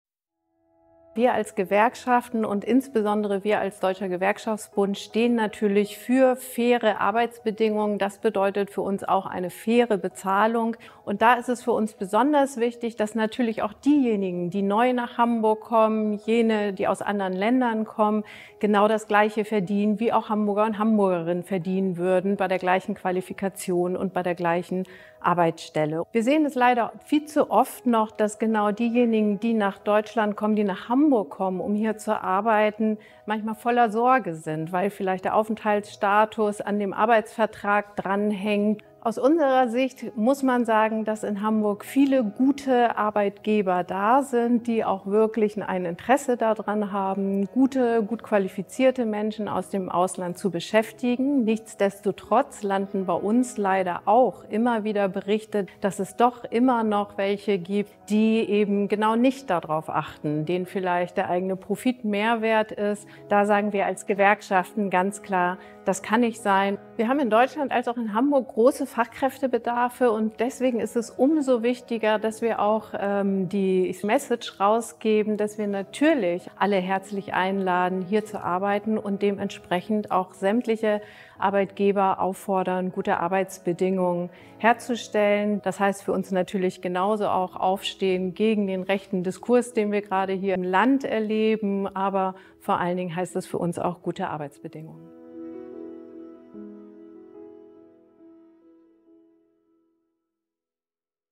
Aktion „Faire Migration – Fair Pay“ des IQ Netzwerks Hamburg: Tanja Chawla, Vorsitzende DGB-Hamburg
Tanja Chawla ist Vorsitzende des Deutschen Gewerkschaftsbundes Hamburg und spricht sich deutlich für faire Bezahlung aus. Das Video-Statement ist Teil der Aktion „Faire Migration – Fair Pay“ des IQ Netzwerks Hamburg.